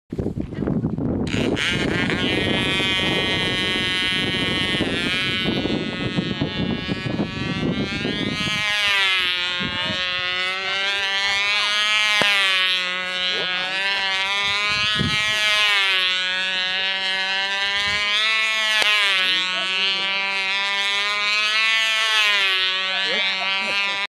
steve balloon bursting
Steve Blades taking part in the balloon bursting event at South Bristol MAC's fun day on 28th July 2012. Steve was using the  Linesman model with a PAW .09 motor.